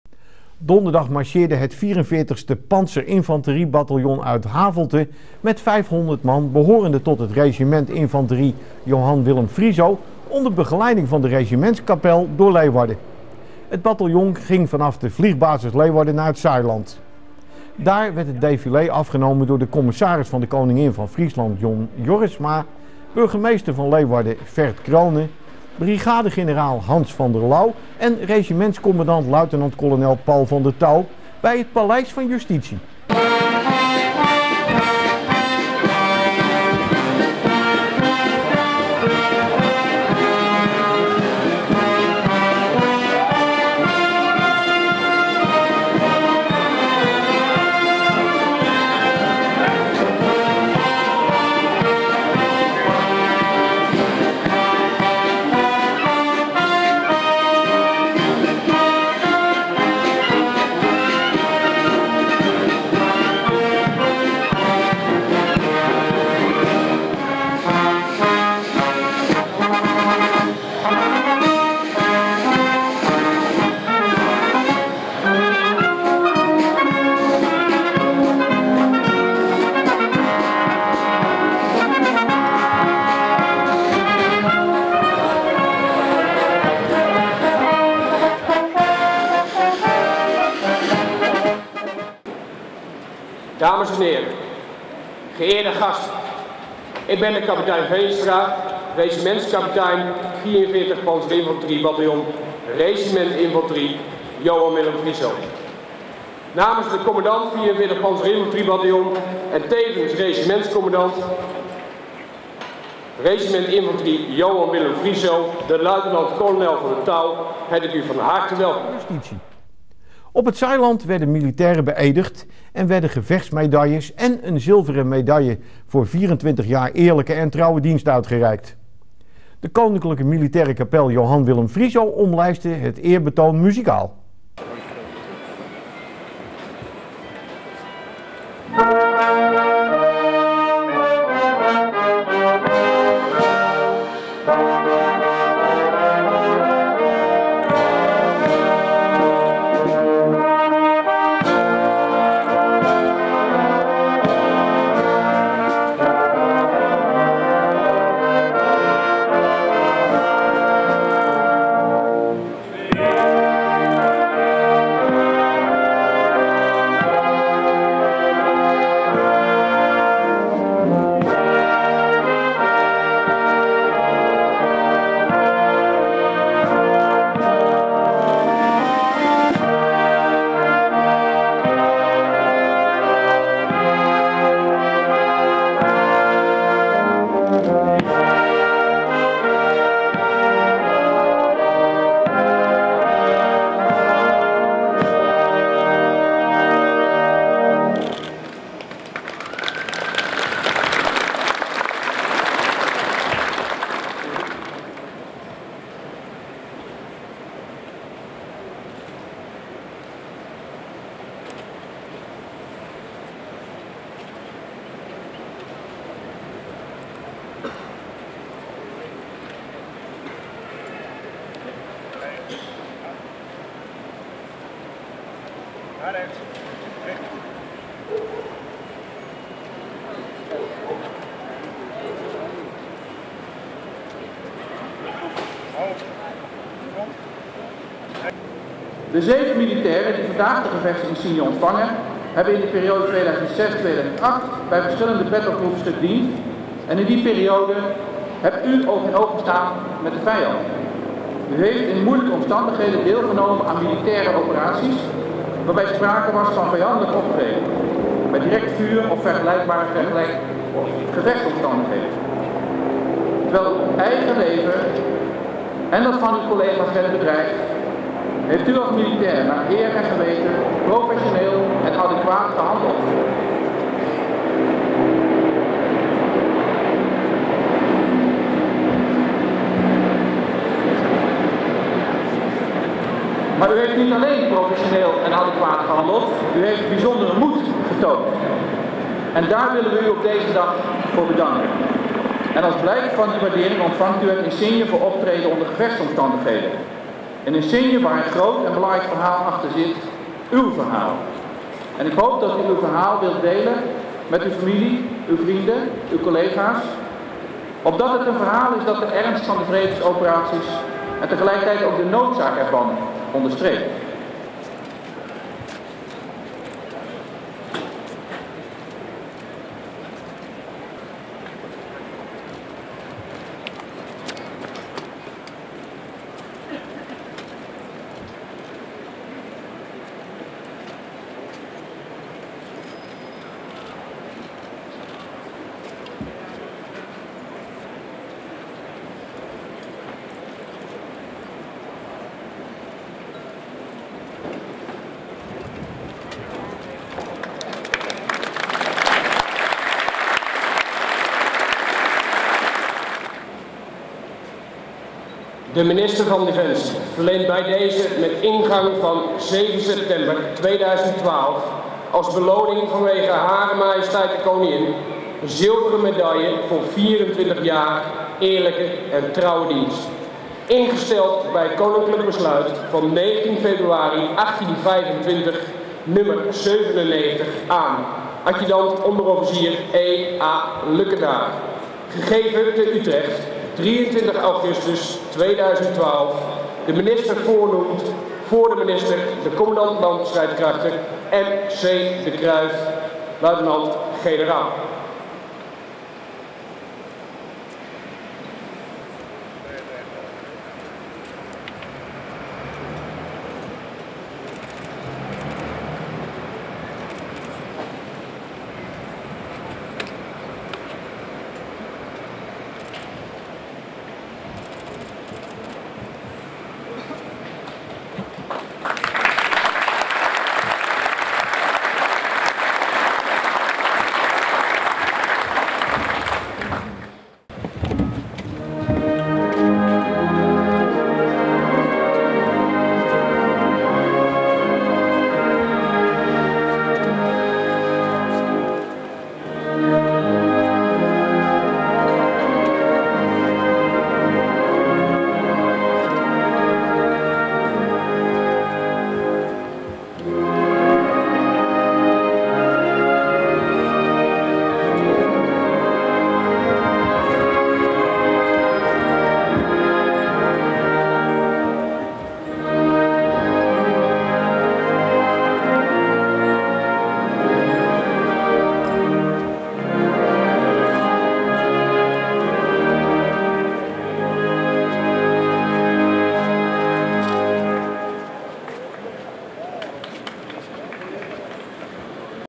GPTV: Militaire eerbetonen op het Zaailand.
Donderdag marcheerde het 44e Pantserinfanteriebataljon uit Havelte met 500 man, behorende tot het Regiment Infanterie Johan Willem Friso, onder begeleiding van de Regimentskapel door Leeuwarden. Het bataljon ging vanaf de vliegbasis Leeuwarden naar het Zaailand.
Daar werd het defilé afgenomen door Commissaris van de Koningin van Friesland John Jorritsma, burgemeester van Leeuwarden Ferd Crone, brigadegeneraal Hans van der Louw en regimentscommandant luitenant-kolonel Paul Van der Touw bij het paleis van Justitie.
Op het Zaailand werden militairen beëdigd en werden gevechtmedailles en een zilveren medaille voor 24 jaar eerlijke en trouwe dienst. De Koninklijke Militaire Kapel Johan Willem Friso omlijstte de eerbetonen muzikaal.